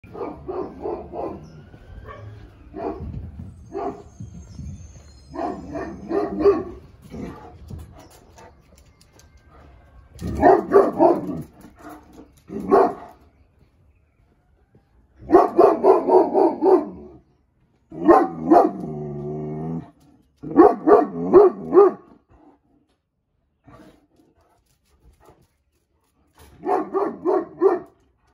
My dog
This is Coral, she’s an Irish Wolfhound, she like food, she’s three years old, and she weighs 185 pounds. She like to bark at people and to go to the dog park. She would never hurt anyone on purpose and she doesn’t know her own size.